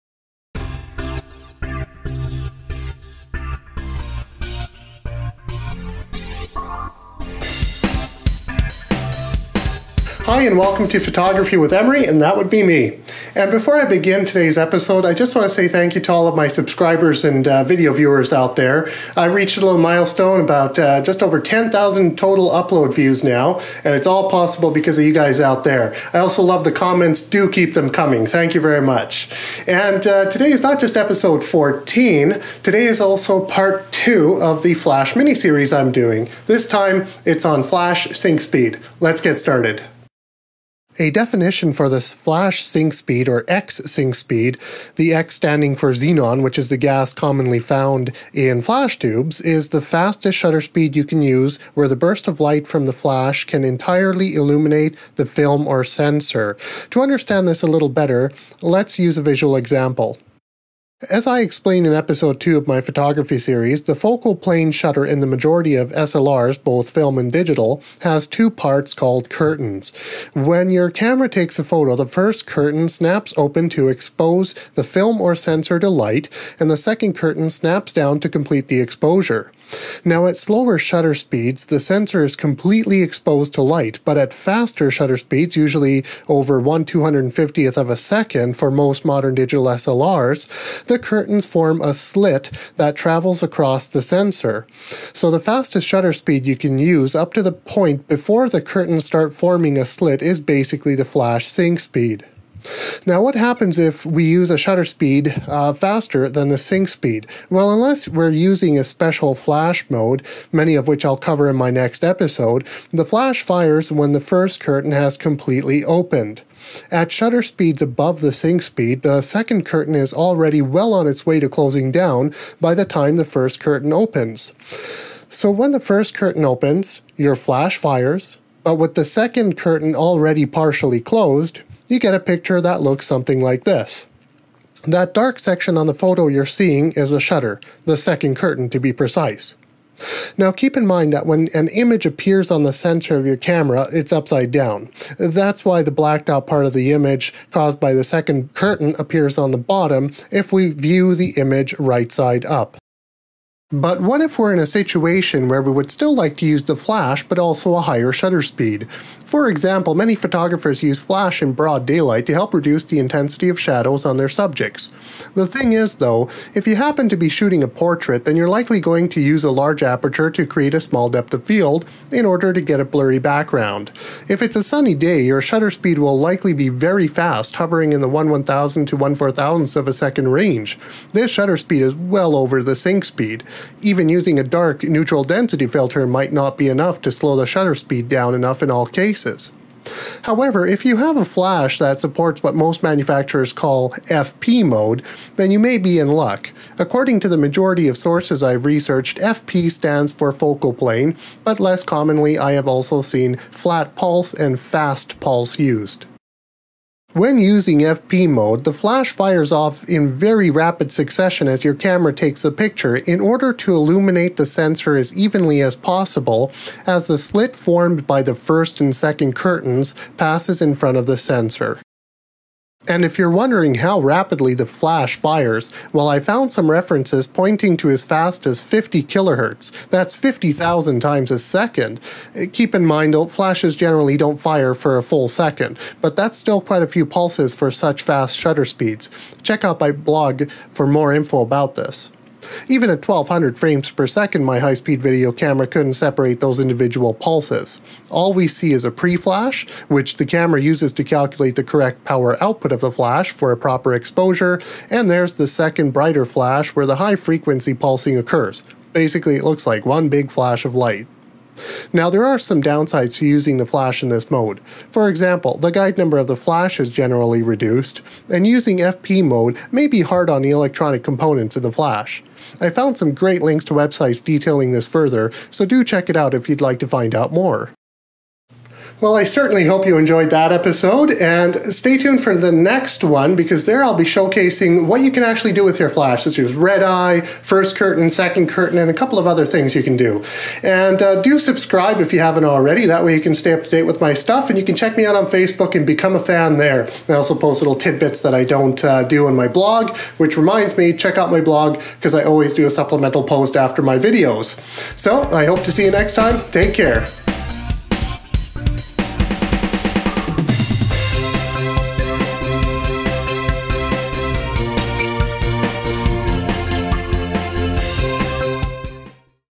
Flashes Part 2: Sync Speed (X-Sync) - Photography with Imre - Episode 14
Episode 14 of my photography series focuses on the flash sync speed; this is the second part of the flash mini-series. You'll also want to check out my blog - as it's supplemented with additional information and links to sites describing this topic in detail.